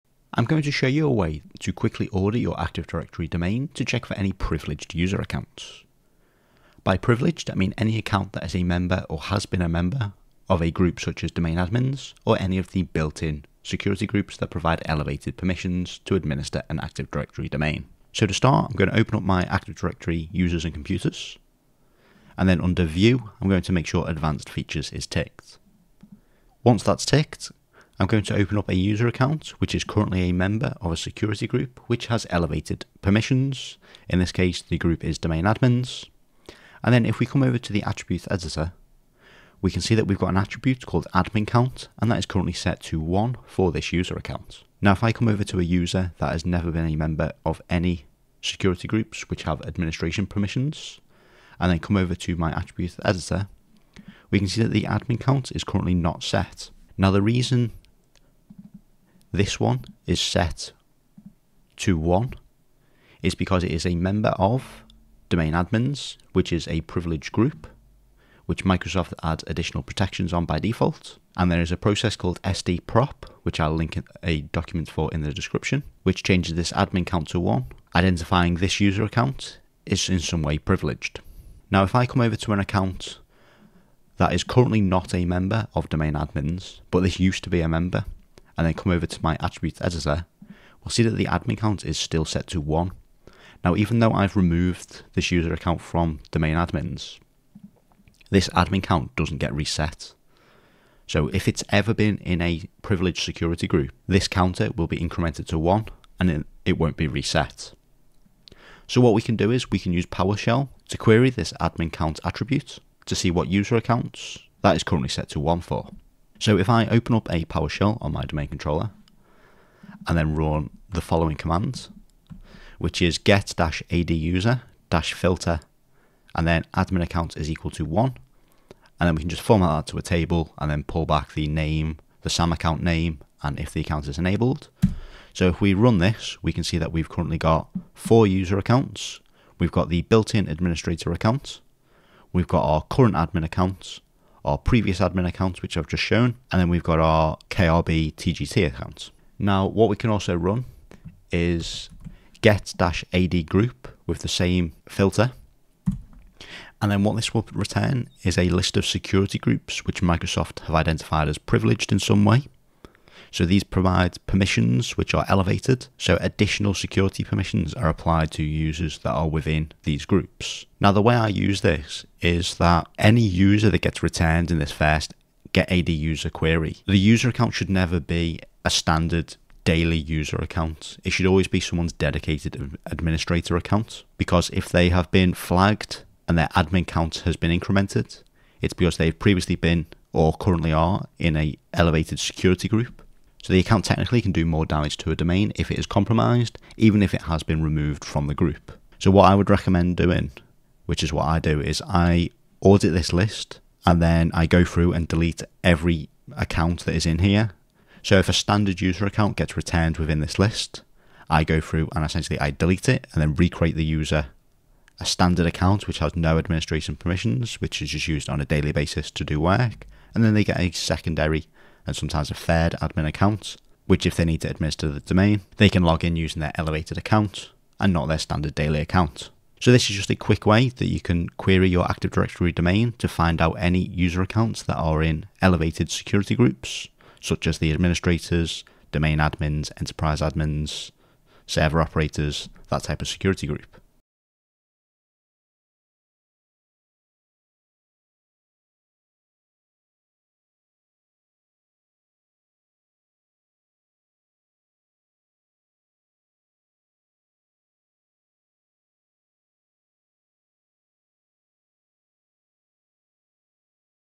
How to find privileged Active Directory Accounts
Learn how to find privileged Active Directory account by using the adminCount attribute. In this example, I show you how to use PowerShell to return a list of user accounts that have had their adminCount attribute changed to 1. User account with an adminCount value of 1 indicate that the user account is either a member of a privileged security group, or has previously been a member of a privileged security group.

Get-ADUser -Filter "adminCount -eq 1" | ft name, samaccountname, enabled

Get-ADGroup -Filter "adminCount -eq 1" | ft name